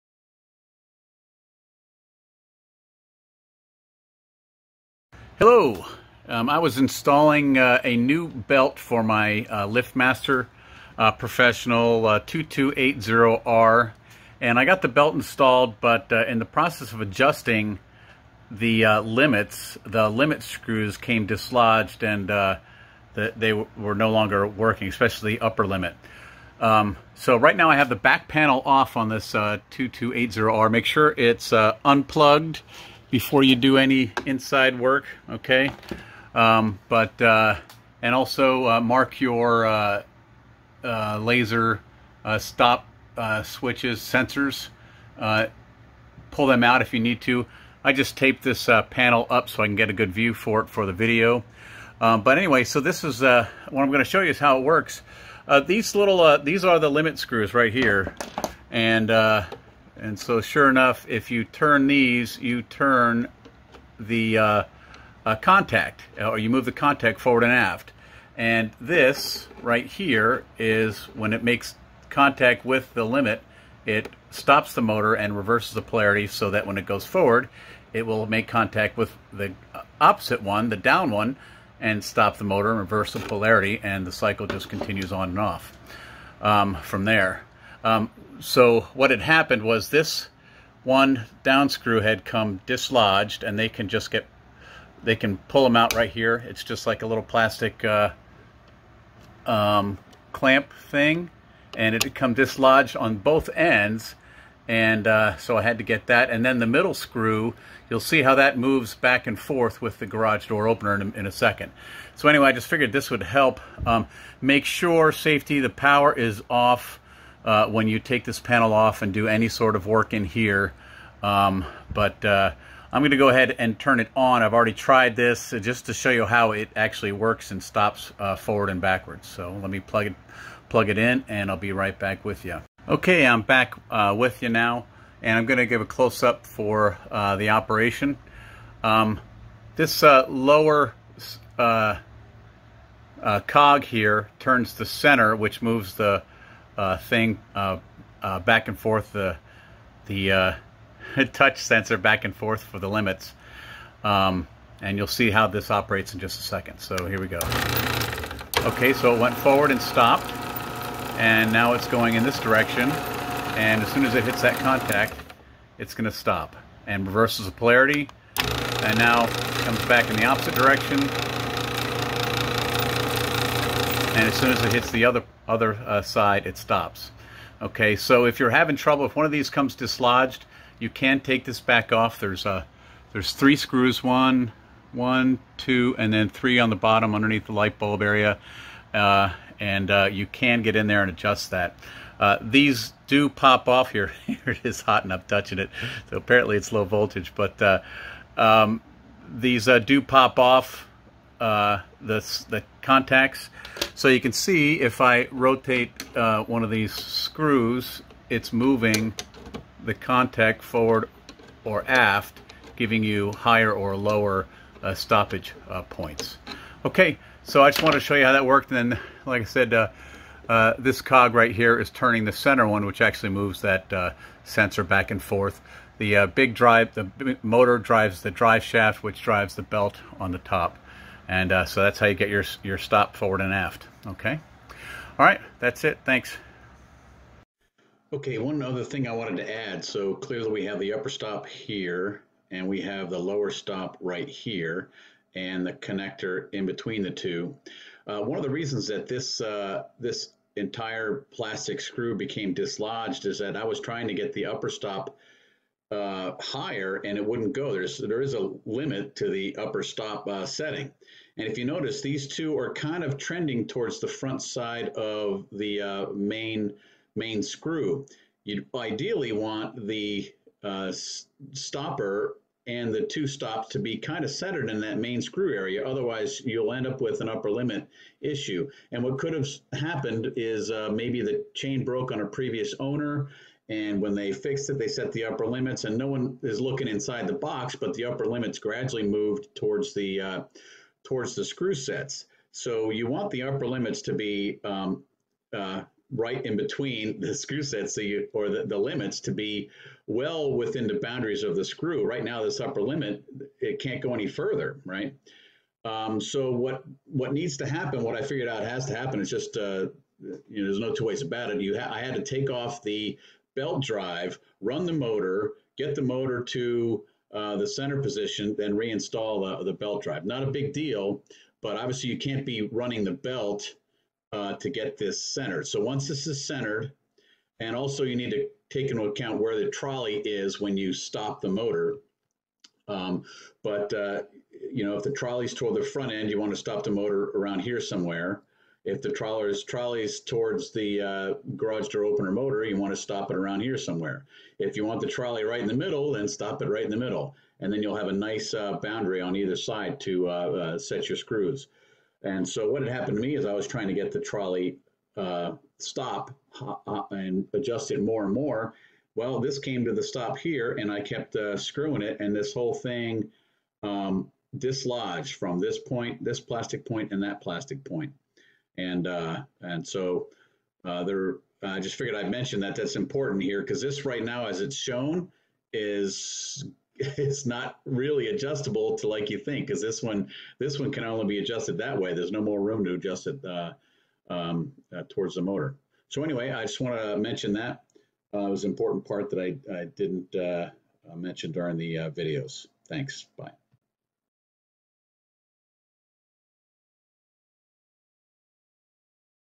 Limit Switch Operation inside a Liftmaster 2280R Garage Door Opener
In the process of replacing my garage-door drive belt, the up limit adjusting screw became dislodged and stopped working. My to my pleasant surprise, the inner workings of this mechanism were not very complex and very easily repairable.  So this video is for those who find that the limit adjusting screws have come dislodged/loose inside the unit and/or have stopped working.